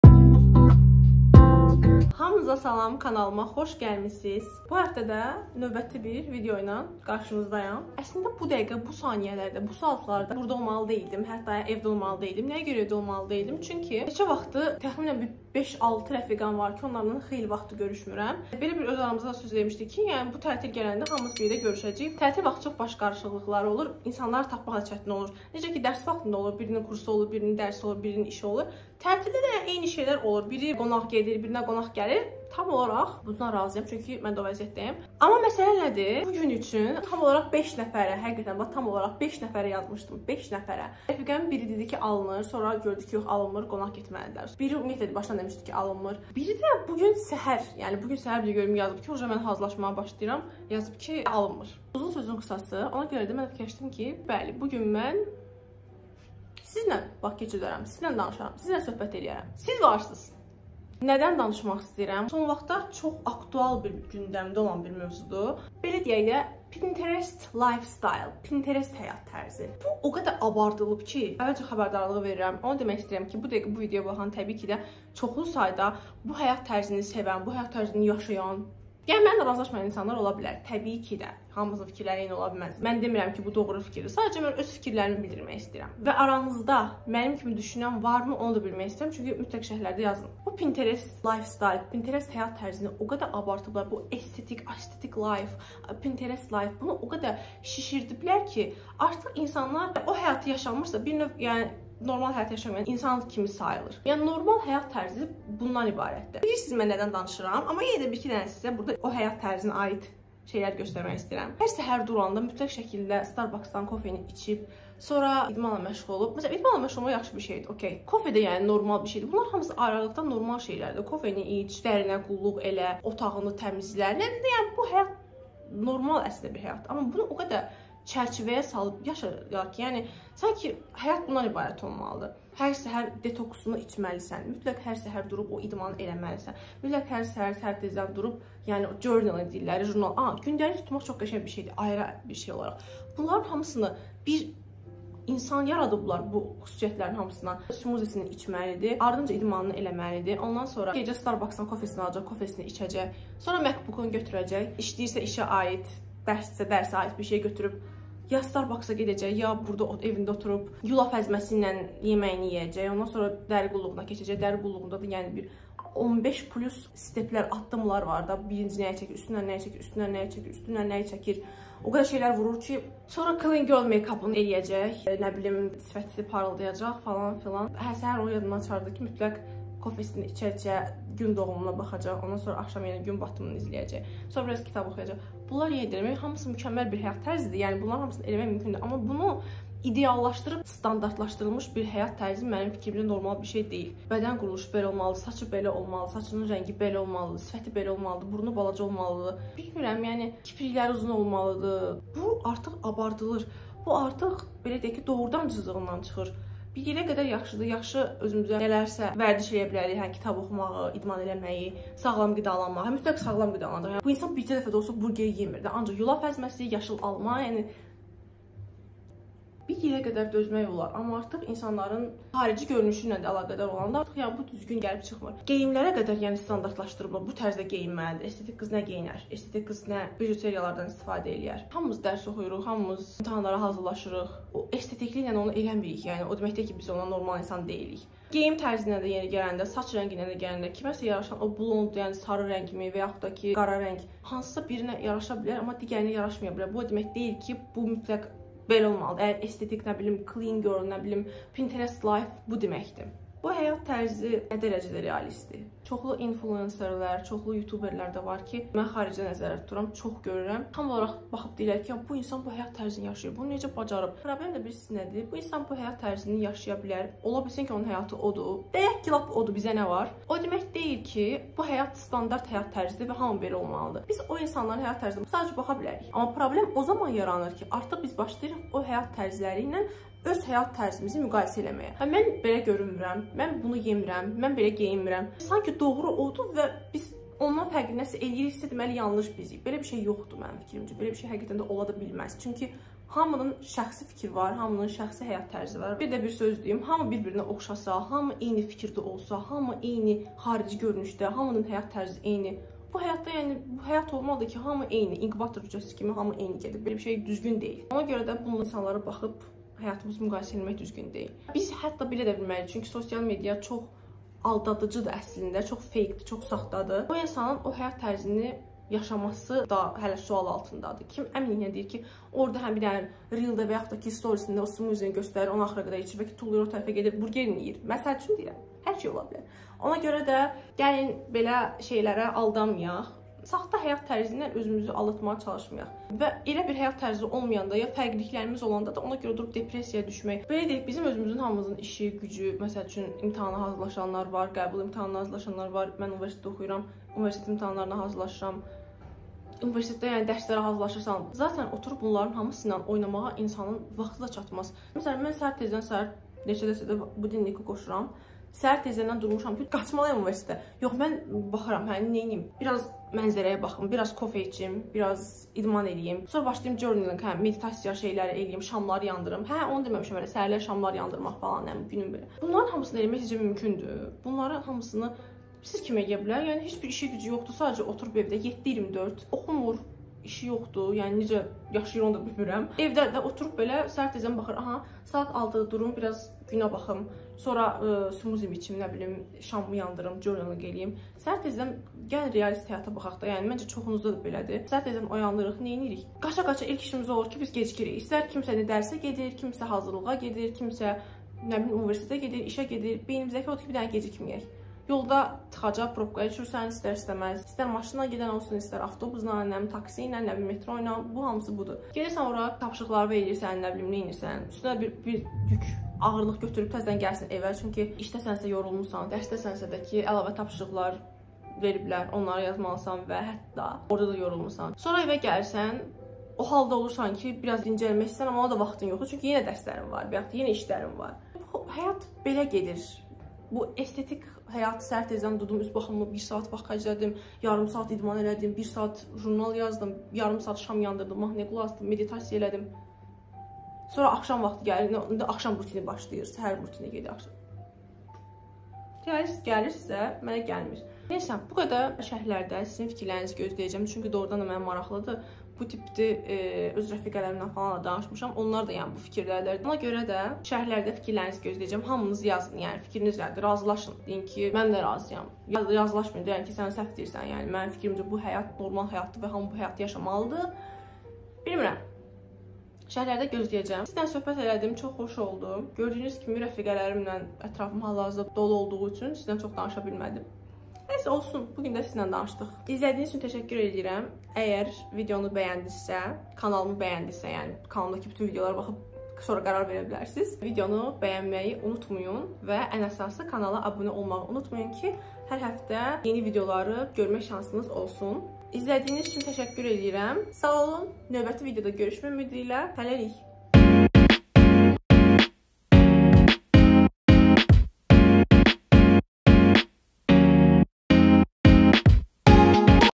“Pinterest” qızı. Aesthetic həyat, clean girl makeup.| Gəlin söhbətləşək
Bu videomda bu zamanlarda çox məşhur olan bir mövzudan danışmaq istədim. Estetik həyat, pinterest həyat tərzi, pinterest qızı olmaq.
Abunə olsanız çox sevinərəm:)